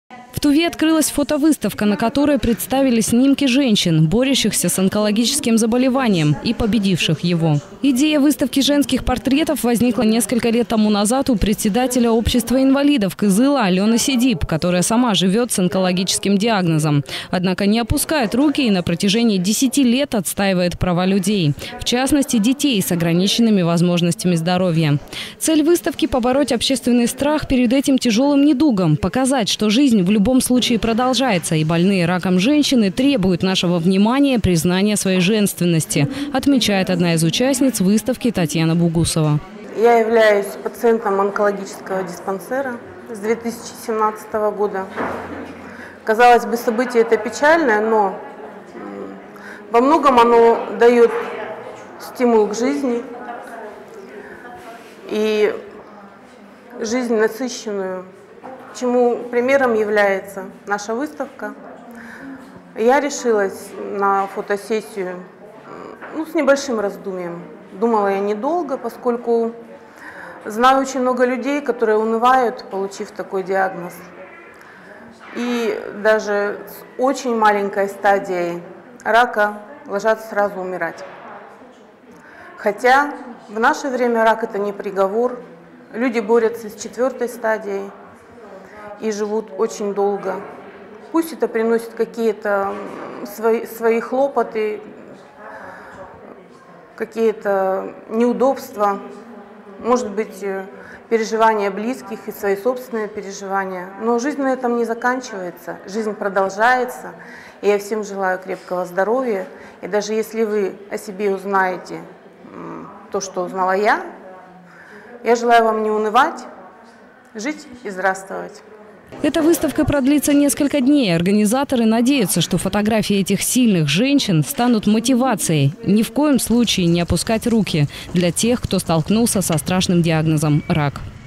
Жизнь продолжается
В Туве открылась фотовыставка, на которой представили снимки женщин, борящихся с онкологическим заболеванием и победивших его.